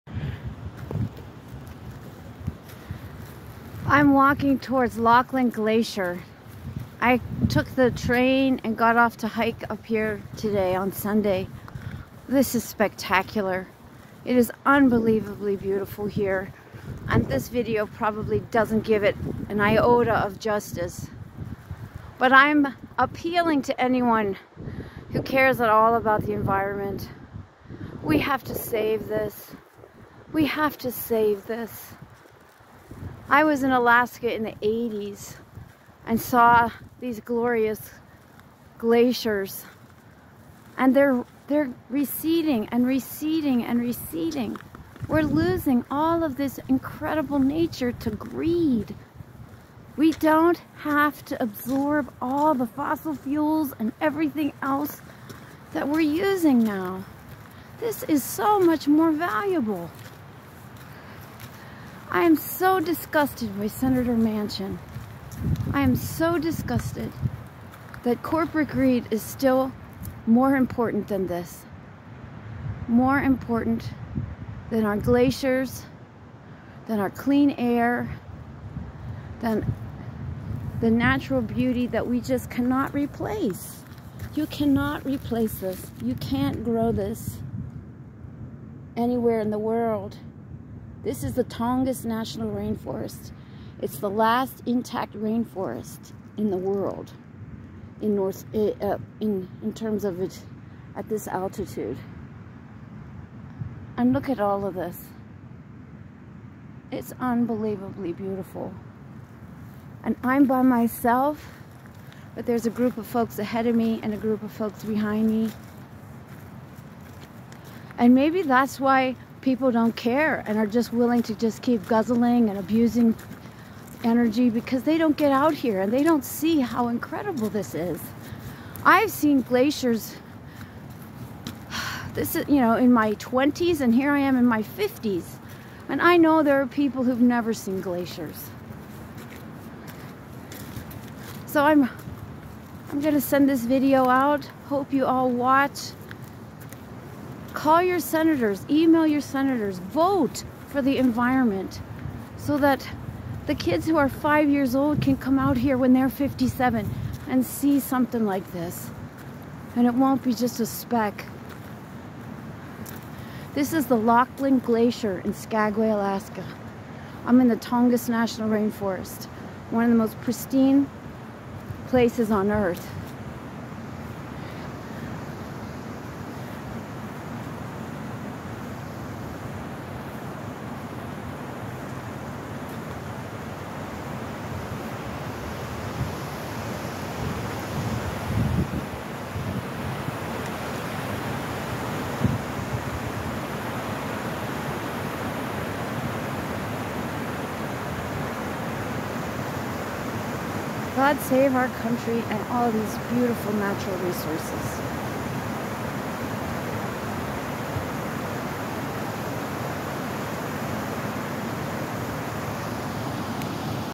Laughton Glacier, Skagway, Alaska
Spectacular hiking in Skagway, Alaska